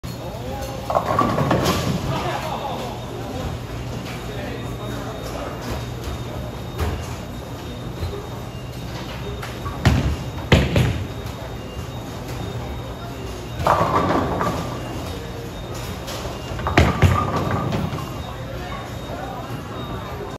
Kenny (5yo) on his first bowling league
Adult/Youth pair teams at Schwoegler’s